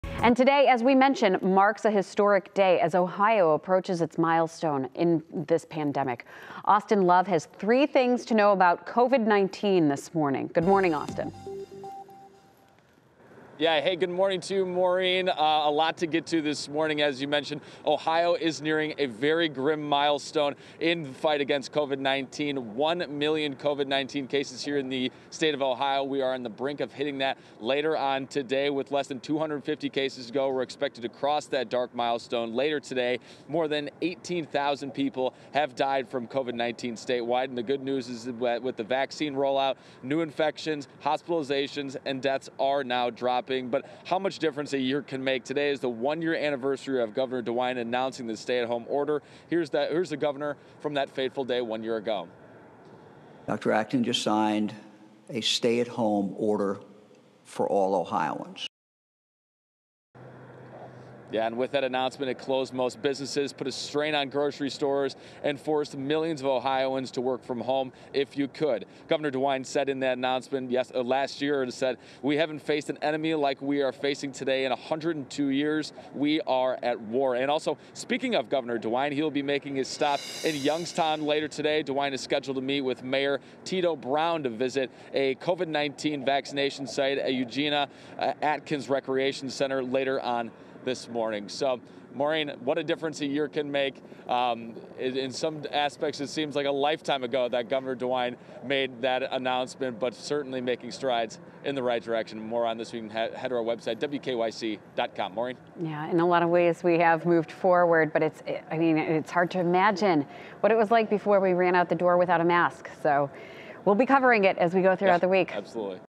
Ohio to reach 1 million COVID-19 cases
A little more than one year ago, Ohio reported its first three coronavirus cases back on March 9, 2020. Now, 378 days later, that number is expected to reach 1 million cases when the state’s latest data is reported at 2 p.m. Monday.

Ohio’s current tally has hit 999,750 after reporting an additional 931 cases Sunday. There have also been more than 18,300 deaths throughout the state.